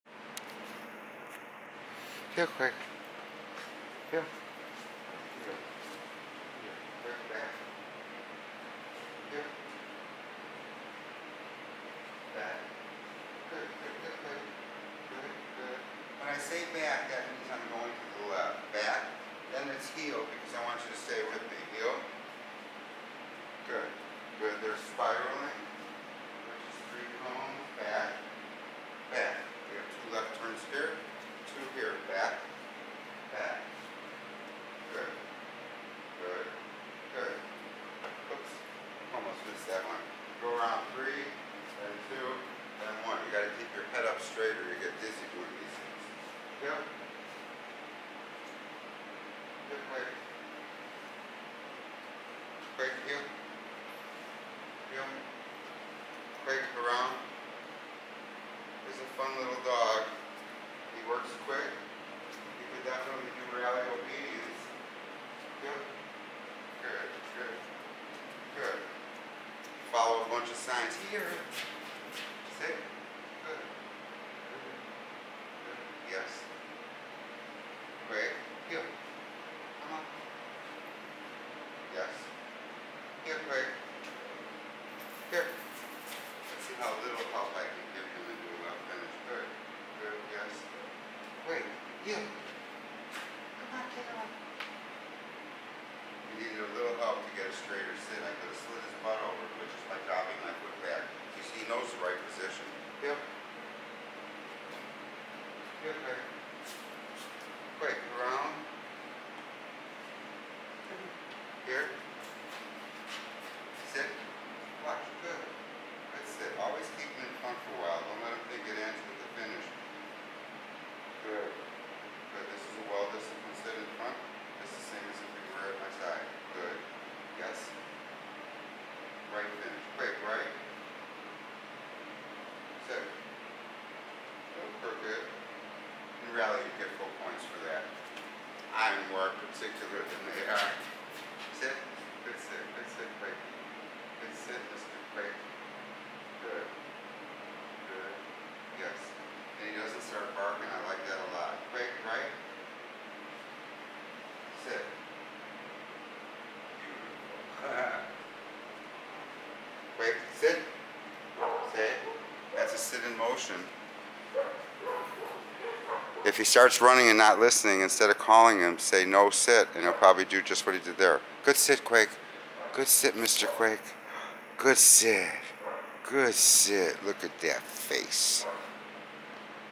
Allan Ross Dog Training
Quake is a 1.0 year old Cavalier King Charles in returning Board and Train.  Here we practice obedience in the Canine College with cones.